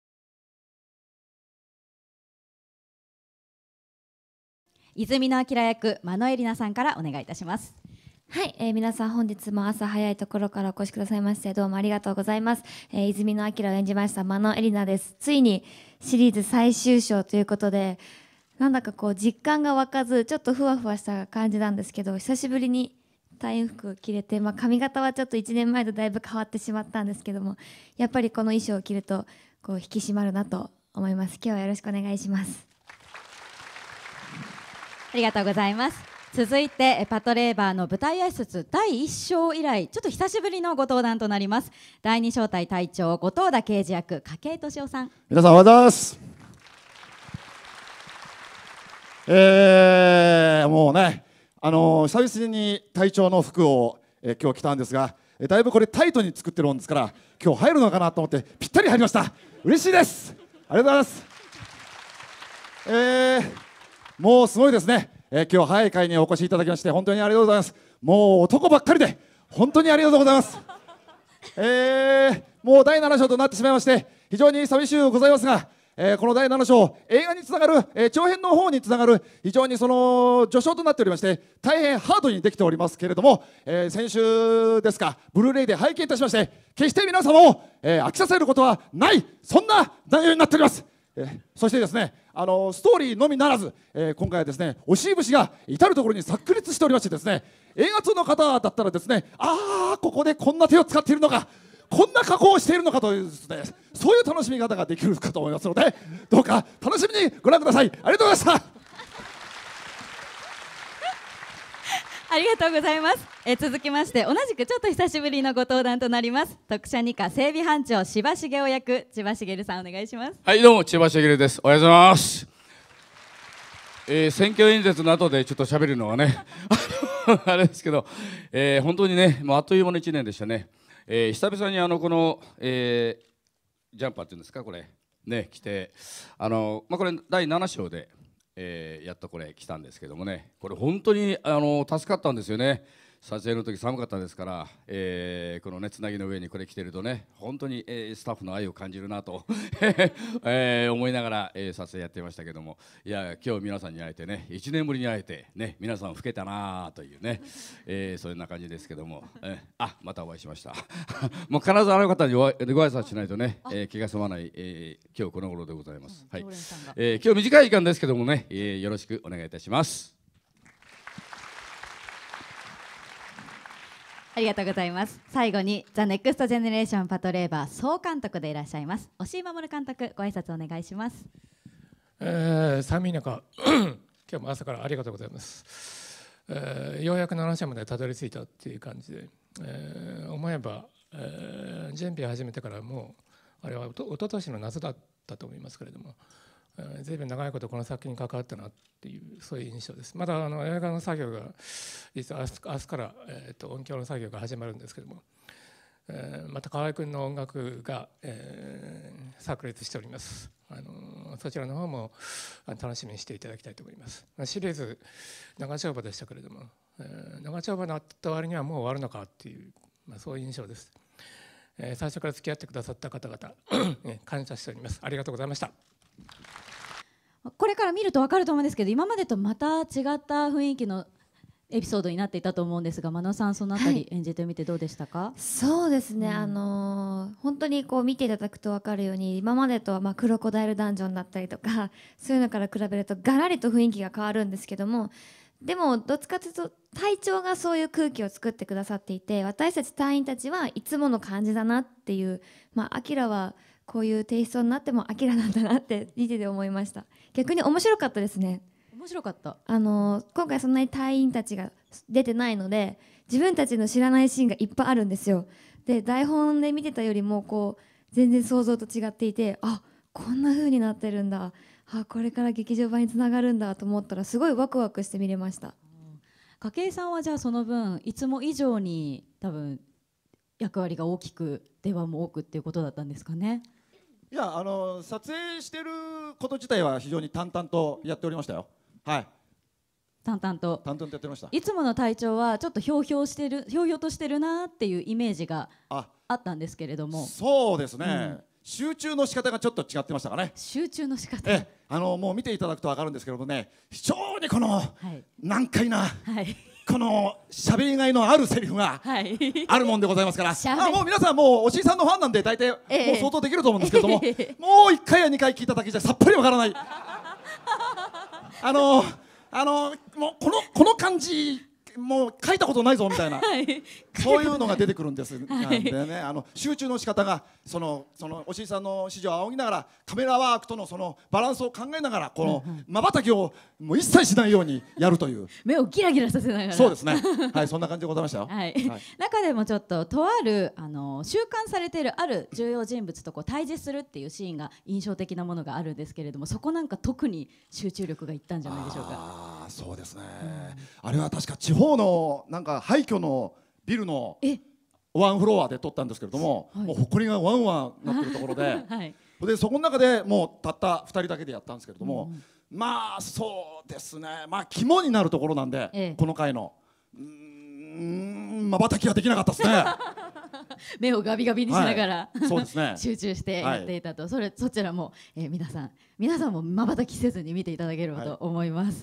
真野恵里菜、久々の役衣装で登場！映画「THE NEXT GENERATION パトレイバー 第7章」初日舞台あいさつ1 movie The Next Generation -Patlabor
「機動警察パトレイバー」シリーズの実写化プロジェクトの最終章にあたる「THE　NEXT　GENERATION　パトレイバー」第7章の初日舞台あいさつが1月10日、東京都内の映画館で開催され、押井守監督や女優の真野恵里菜さん、筧利夫さん、千葉繁さんが登場した。

　「機動警察パトレイバー」は、歩行式の作業機械・レイバーが実用化された近未来を舞台に、レイバー犯罪に立ち向かう警視庁の特科車両2課中隊（特車2課）の活躍や隊員の日常を描いたSF作品。1988～94年に「週刊少年サンデー」（小学館）でゆうきまさみさんのマンガが連載され、89～90年にはテレビアニメも放送された。

　今回の新作は、押井監督が総監督・脚本を担当。全7章のシリーズがイベント上映中で、第7章は、1993年に公開された劇場版アニメ「機動警察パトレイバー 2 the Movie」のアフターストーリーとなるエピソード12「大いなる遺産」と、「シリーズ総集編」で構成されている。人気キャラクター・南雲しのぶが登場するほか、5月に公開予定の長編劇場版「THE NEXT GENERATION パトレイバー 首都決戦」への布石となる内容も盛り込まれる。

The greetings on stage in the premiere of the movie "The Next Generation -Patlabor- Part 7", which is the final part of the live-action adaptation project of the series "Mobile Police Patlabor", was held in a cinema in Tokyo on January 10th, with the presence of director Mamoru Oshii, actress Erina Mano, Toshio Kakei and Shigeru Chiba.